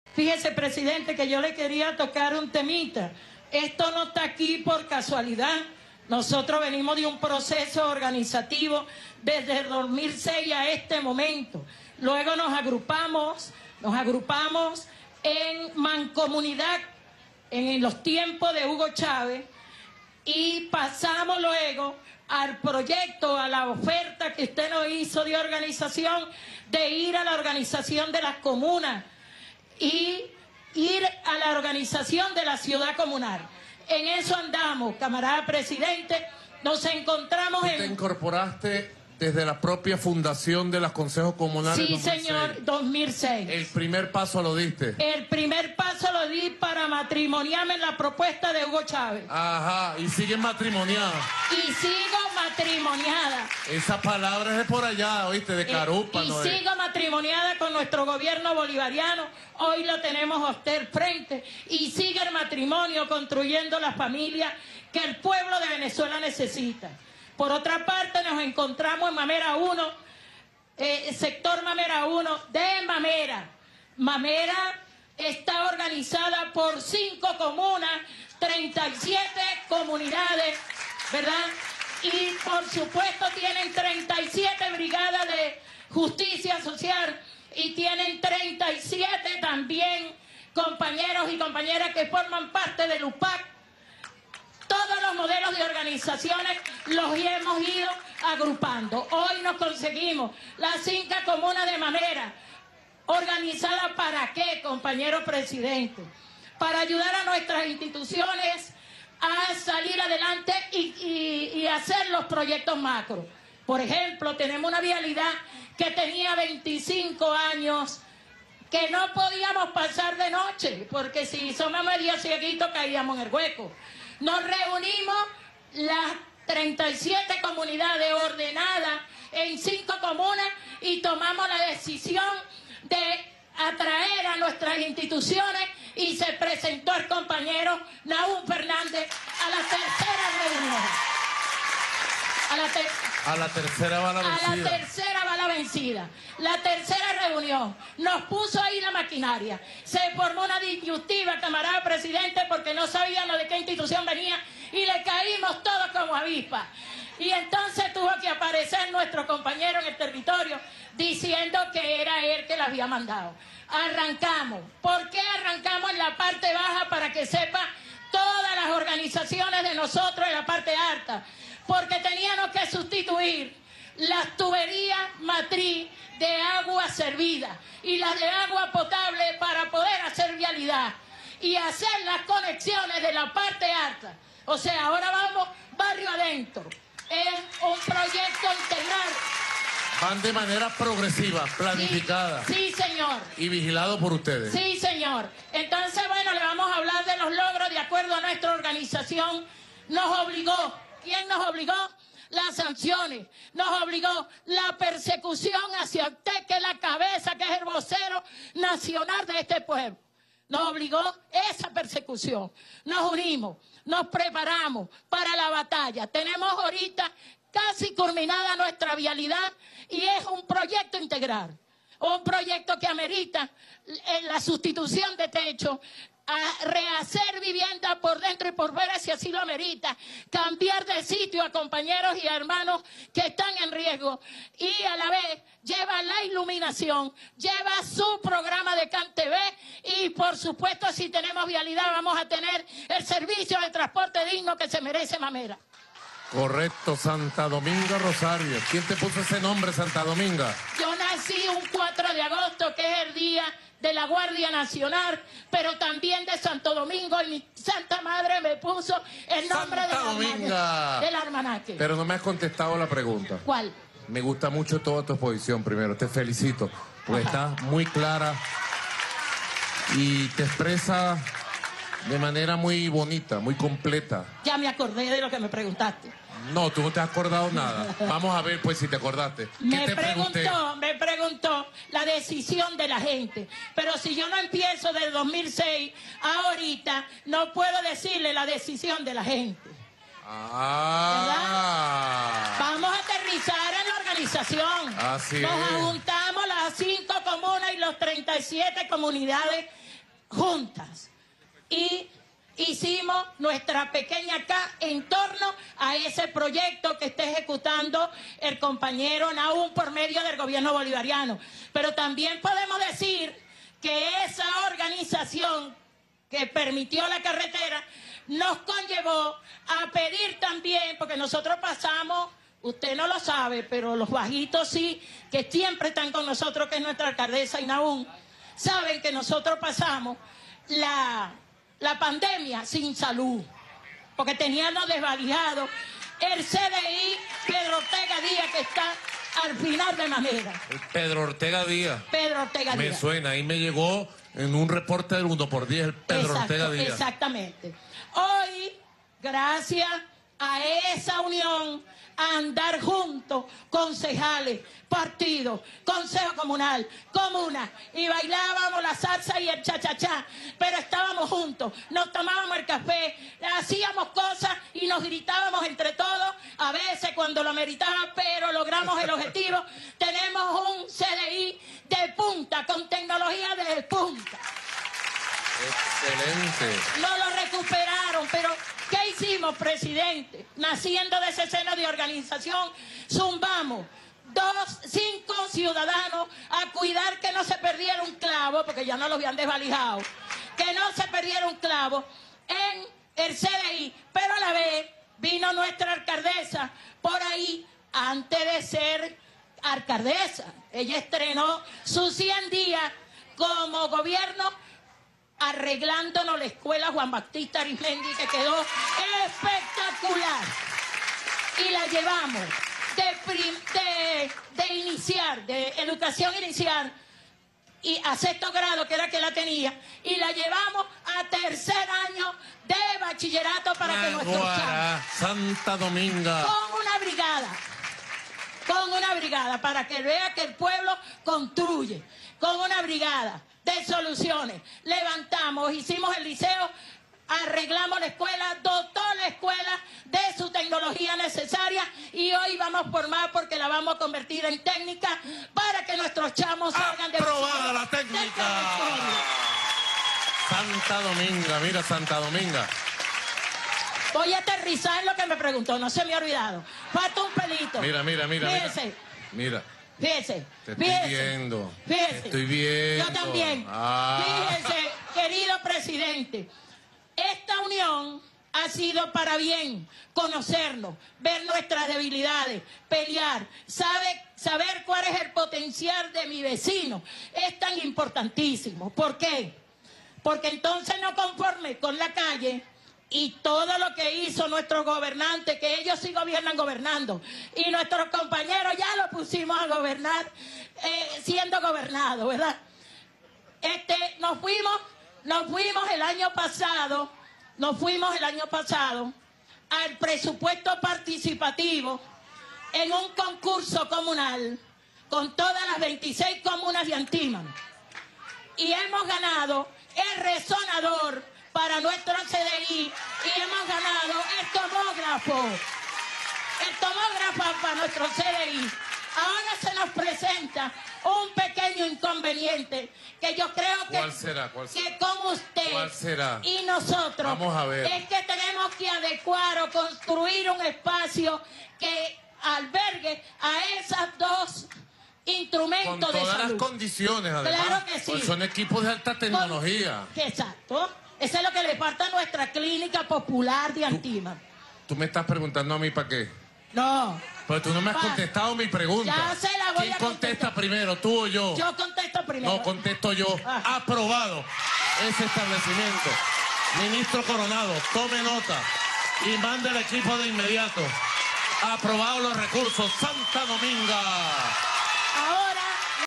Señora de Mamera conversa con Maduro sobre lo difícil que fue arreglar las calles (pero lo lograron)
Quise dejar este video del acto del Presidente Nicolás Maduro este 25 de abril de 2024 con comunas y consejos comunales en Mamera, oeste de Caracas. Allí una señora, Santa Dominga Rosario, organizadora de los consejos comunales de la zona, habla con Maduro y le cuenta lo complicado que fue para ellos organizarse para lograr reparar las vías, que tenían 25 años dañadas. Pero finalmente lo lograron.

En resumen:

✅Cuentan que tuvieron que unirse todos los consejos comunales y comunas de Mamera y convocar a reuniones a las autoridades (Jefe de Gobierno y alcaldesa);

✅que fue a la tercera reunión que finalmente el Jefe de Gobierno asistió a la reunión;

✅que luego pusieron las maquinarias pero nadie sabía quién las puso (desorganización de las instituciones) y tuvieron que formar lío para que los responsables aparecieran;

✅que se iniciaron los trabajos pero fueron complejos porque también había que cambiar las tuberías de aguas servidas y de aguas blancas y por eso hubo que empezar en la parte baja del barrio para luego ir hacia arriba, y la gente de arriba estaba molesta;

✅que siguieron formando lío y usando las diferentes formas de organización y presupuesto y lograron que la escuela de Mamera, que sólo llegaba a 6to grado, llegara a 3er año y ahora están bregando para que sea escuela técnica;

✅que bregaron por aquí y por allá y lograron que les arreglaran el CDI, que había quedado destruido desde la pandemia, pero tuvieron que poner 5 personas de su comunidad a cuidarlo porque unos malandros lo desvalijaban

✅que metieron más presupuestos y solicitudes y lograron el resonador y el tomógrafo 😐

✅ que ahora están metiendo proyectos para arreglar los techos de las casas, llaman a Barrio Tricolor para que les hagan las paredes y fachadas y las arreglen por dentro, etc. Ojalá se lo hagan a todas las casas y no sólo a las de un grupo.

En fin, lo que quería llamarles la atención es:

1) Lo complicado que es lograr, aún en este gobierno chavista, que las instituciones le paren a la  gente, y que la gente de Mamera lo logró porque es perseverante, unida, peleona pero diplomática, resteada, etc.

2) ¿Ustedes creen que estas personas, de un barrio bien arriba allá en Mamera y Antímano, van a creen que una señora como la MCM o un señor como Edmundo el de las guacamayas, que sólo se ha atrevido a sacarse fotos en una quinta en el sureste de Caracas, van a resolver sus problemas? Si tienen que PARIR para que las autoridades actuales les solucionen los problemas, imaginen cuánto tendrían que parir con una MCM, que no dudará de echarle la policía encima si se ponen cómicos?

3) Coño, el gobierno tiene que buscar formas de servir a la gente sin que la gente tenga que parir tanto, ¿no? Porque no en todos lados hay una señora como Santa Dominga, que se ve que es bien arrecha, resteada y perseverante.

En fin, vean el video.

USDT o Bitcoin (BEP20): 0xef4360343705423a24119205eeb1de2a10fd23f4